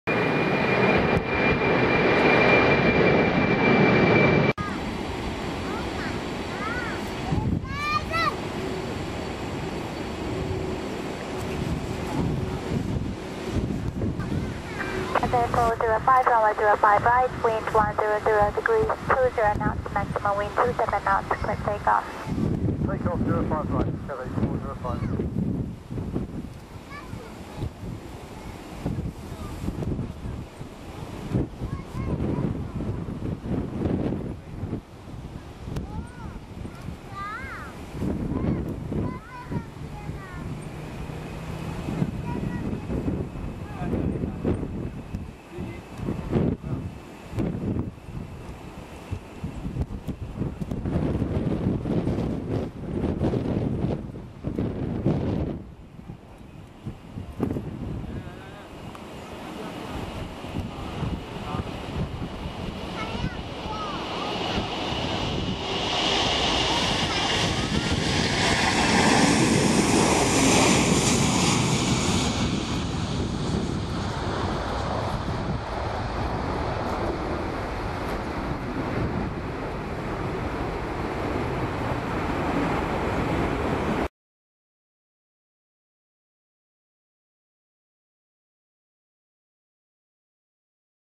White tail MD-11F N304MS Largest glider
Delivered to EVA MD-11 as N103EV/B-16103, converted to freighter 08-2004, became B-2176 Shanghai Airlines 05-2005, then N304MS 06-2011 GECAS